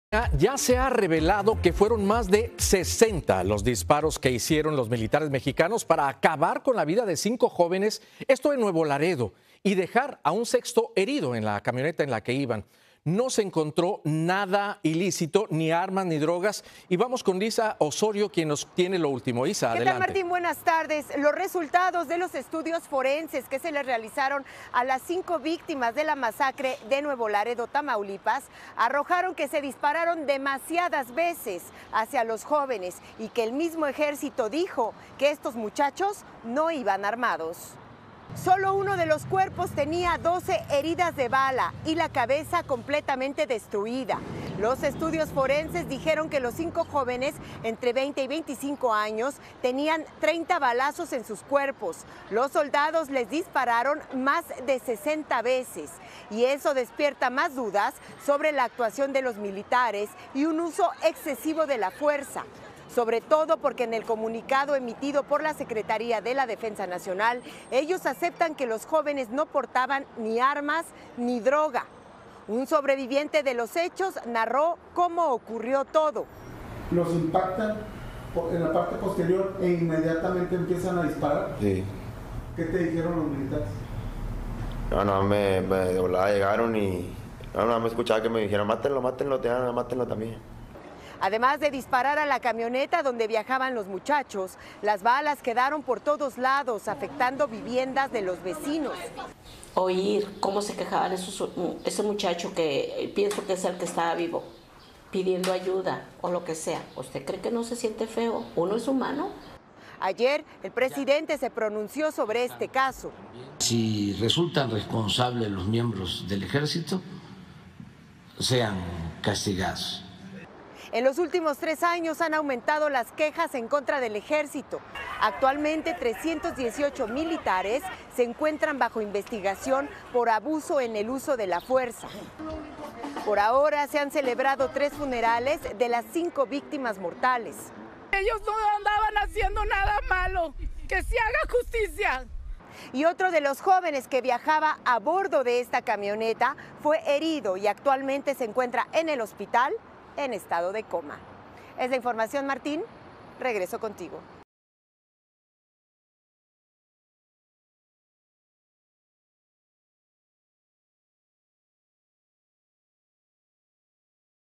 Ejército disparó 60 veces contra jóvenes de Nuevo Laredo | Noticias Telemundo
Video oficial de Noticias Telemundo. Los jóvenes de entre 20 y 25 años fueron alcanzados por 30 balas, dejando a cinco muertos y un sexto en coma inducido en un hospital de Tamaulipas. Las autoridades militares han reconocido que ninguna de las víctimas portaba drogas o armas.